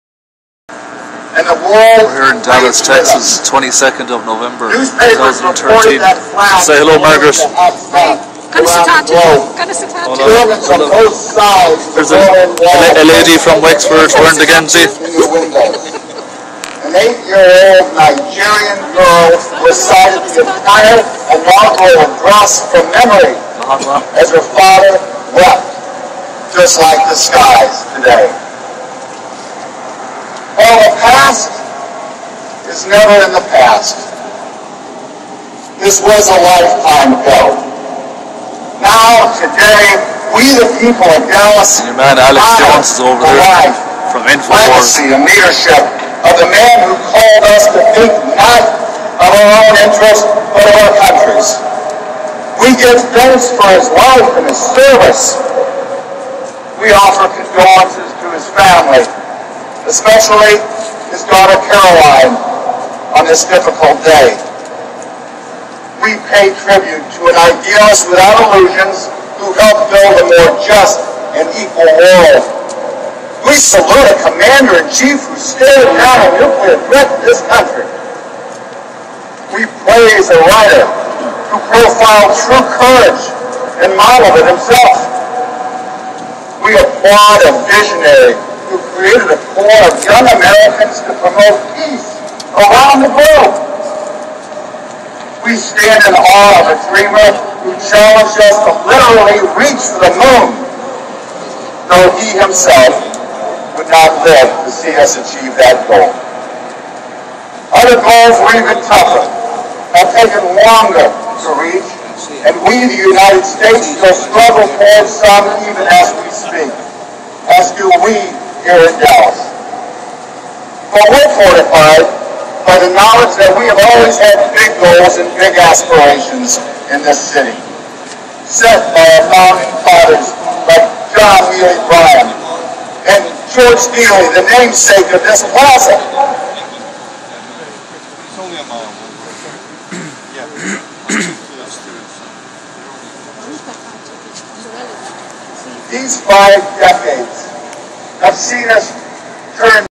JFK : Dallas,TX,50th anniversary on Main st.
A look from Main st,while Mayor Rawlings gives the speech to in essence free Dallas from its association with JFK. for obvious commercial reasons,he had the red & black X's on Elm st erased!! WTF?